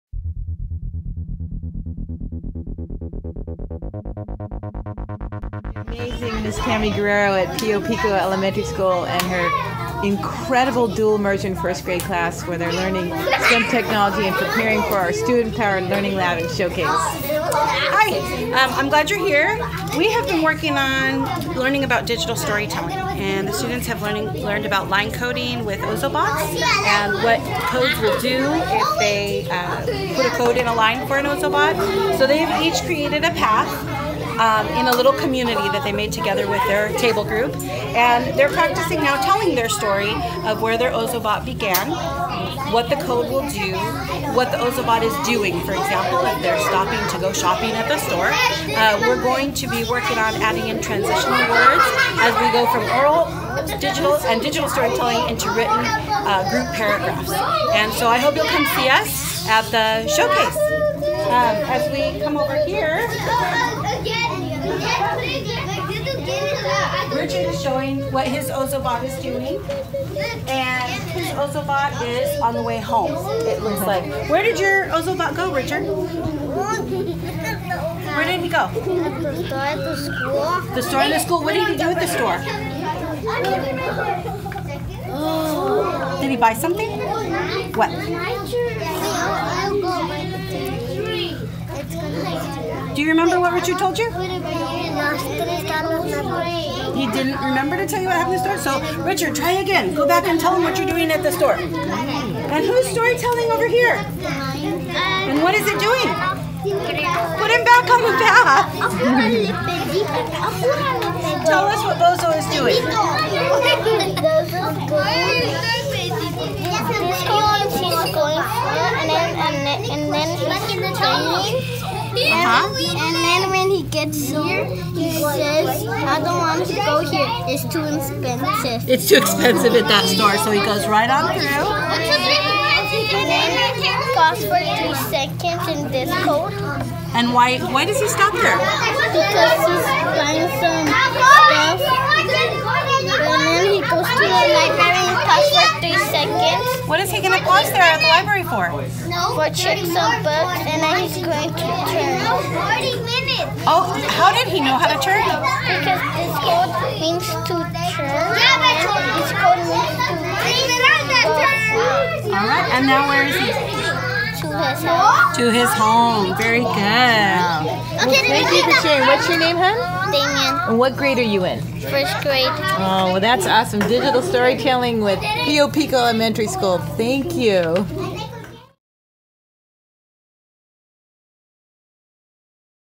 SAUSD Student-Powered Showcase - Ms. Tammy Guerrero Dual-Immersion ES Pío Pico Coding 1st Graders
The Santa Ana Unified School District invited the entire community to the 2017 Student-Powered Learning Lab and Showcase!

The event was held on Wednesday, May 24, 2017, from 4-6PM, at McFadden Institute of Technology in Santa Ana, California.

This free event is open to all SAUSD students, staff, families and community members to attend and see the amazing work of our SAUSD teachers and students.

Are you a teacher who would like to get ideas about how to infuse technology into teaching and learning?

Are you a parent or community member who would like to see our students' purposeful use of technology?

Do you want to have fun participating in hands-on learning including demonstrations from local businesses and community partners?